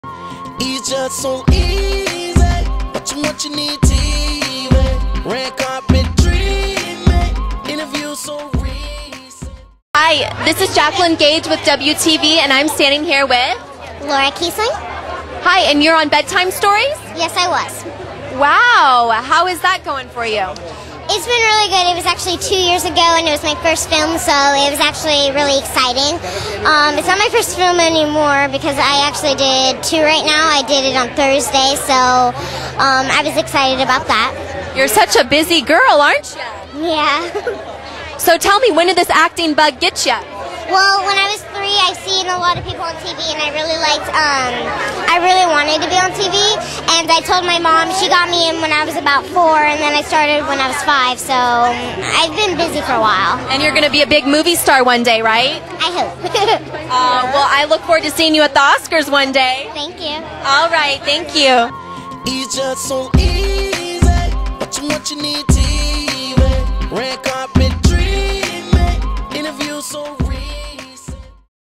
Laura Ann Kesling "Bedtime Stories" 2009 World Magic Awards
Laura Ann Kesling attends 2009 World Magic Awards Oct. 10th